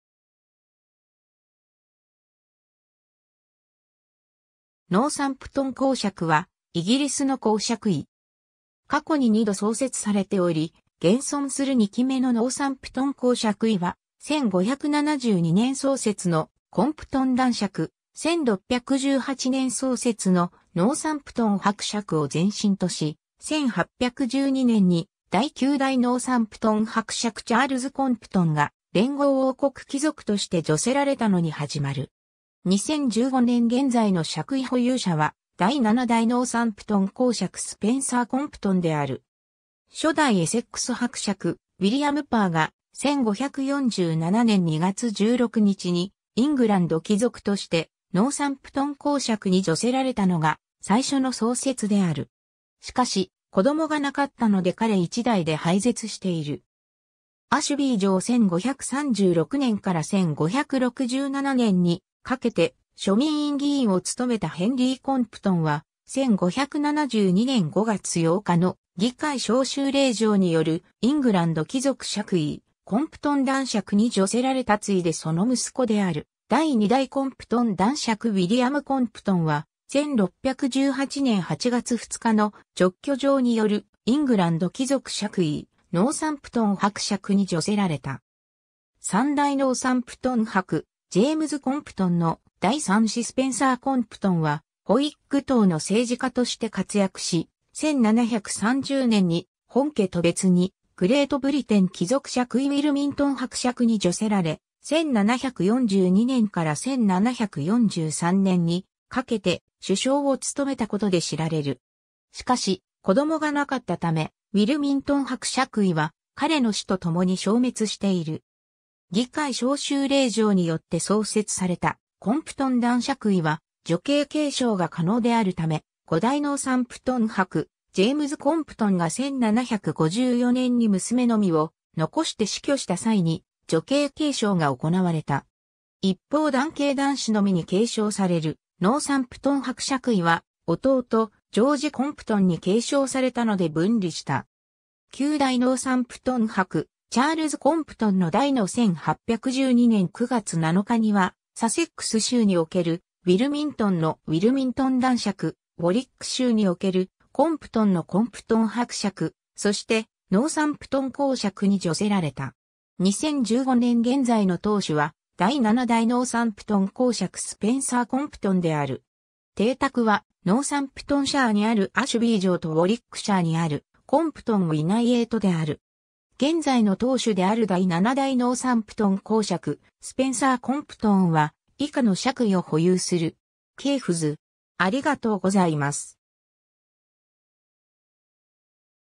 ノーサンプトン侯爵
ノーサンプトン侯爵（英語: Marquess of Northampton）は、イギリスの侯爵位。
 過去に2度創設されており、現存する2期目のノーサンプトン侯爵位は、1572年創設のコンプトン男爵、1618年創設のノーサンプトン伯爵を前身とし、1812年に第9代ノーサンプトン伯爵チャールズ・コンプトンが連合王国貴族として叙せられたのに始まる。
2015年現在の爵位保有者は第7代ノーサンプトン侯爵スペンサー・コンプトンである。
 初代エセックス伯爵ウィリアム・パー(1513–1571)が、1547年2月16日にイングランド貴族としてノーサンプトン侯爵に叙せられたのが最初の創設である。
しかし子供がなかったので彼一代で廃絶している。
 アシュビー城 1536年から1567年にかけて庶民院議員を務めたヘンリー・コンプトン(1538頃-1589)は、1572年5月8日の議会招集令状によるイングランド貴族爵位「コンプトン男爵(Baron Compton)」に叙せられた ついでその息子である第2代コンプトン男爵ウィリアム・コンプトン(-1630)は、1618年8月2日の勅許状によるイングランド貴族爵位「ノーサンプトン伯爵(Earl of Northampton)」に叙せられた。
 3代ノーサンプトン伯ジェイムズ・コンプトン(1622–1681)の第3子スペンサー・コンプトン(1674頃–1743)は、ホイッグ党の政治家として活躍し、1730年に本家と別にグレートブリテン貴族爵位ウィルミントン伯爵に叙せられ、1742年から1743年にかけて首相を務めたことで知られる。
しかし子供がなかったため、ウィルミントン伯爵位は彼の死とともに消滅している。
 議会招集令状によって創設されたコンプトン男爵位は女系継承が可能であるため、5代ノーサンプトン伯ジェイムズ・コンプトン(1687–1754)が1754年に娘のみを残して死去した際に女系継承が行われた。
一方男系男子のみに継承されるノーサンプトン伯爵位は、弟ジョージ・コンプトン(1692–1758)に継承されたので分離した。
 9代ノーサンプトン伯チャールズ・コンプトン(1760–1828)の代の1812年9月7日には「サセックス州におけるウィルミントンのウィルミントン男爵(Baron Wilmington, of Wilmington in the County of Sussex)」、「ウォリック州におけるコンプトンのコンプトン伯爵(Earl Compton, of Compton in the County of Warwick)」、そして「ノーサンプトン侯爵(Marquess of Northampton)」に叙せられた。
 2015年現在の当主は第7代ノーサンプトン侯爵スペンサー・コンプトン(1946-)である。
 邸宅はノーサンプトンシャーにあるアシュビー城とウォリックシャーにあるコンプトン・ウィナイエイトである。
 現在の当主である第7代ノーサンプトン侯爵スペンサー・コンプトンは以下の爵位を保有する。
 系譜図